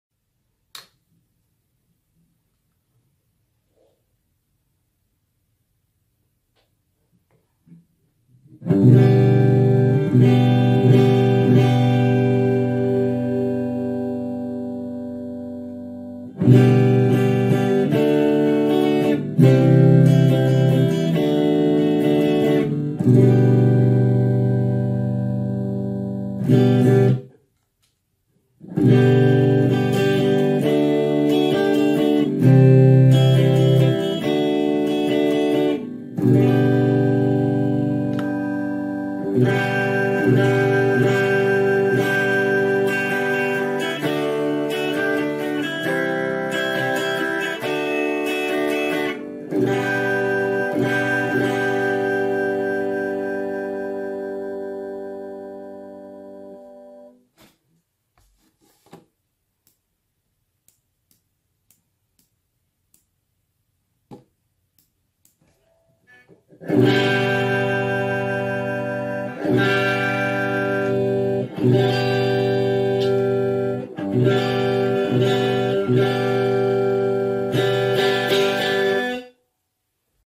Epiphone Les Paul Standard Clean Tones(Warm and Bright Channel)
2010 Les Paul Standard Epiphone
Blackstar Core İD 10 V2
Warm/Bright Channel